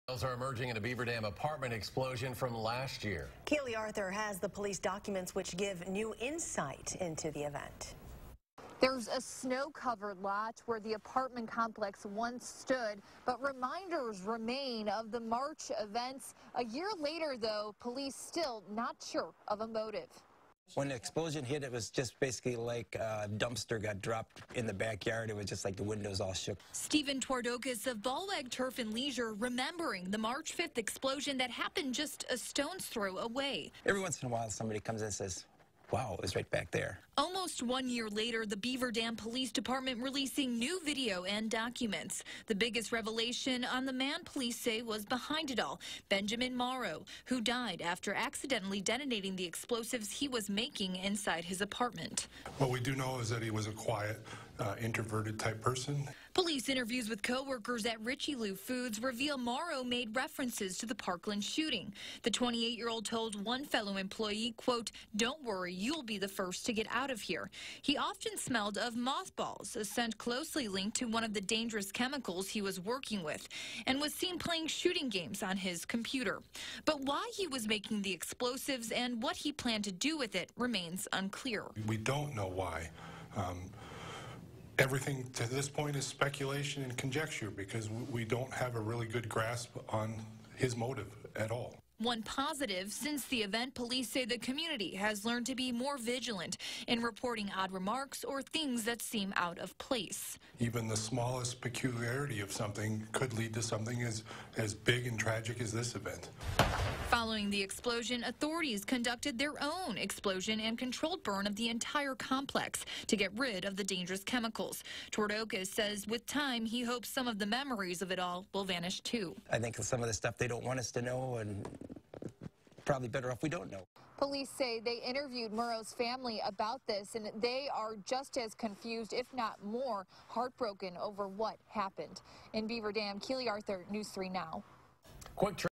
Police reveal more details of Beaver Dam bombmaker
New details about a Beaver Dam apartment explosion are emerging. Nearly a year after the event, the Beaver Dam Police Department has released documents, which give more insight into the man behind the explosion.